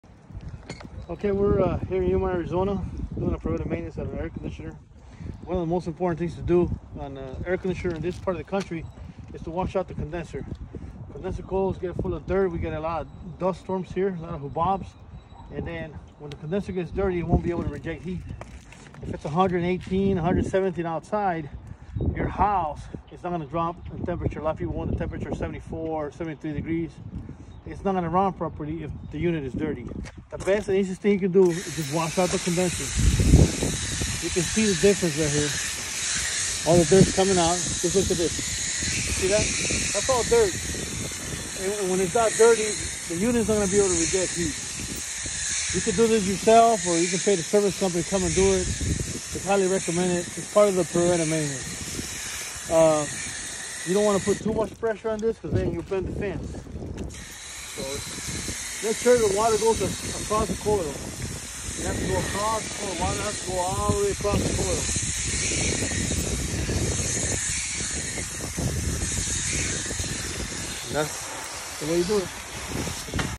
air conditioner cleaning in Yuma Arizona
The importance of cleaning your AIR CONDITIONER coil in Yuma Arizona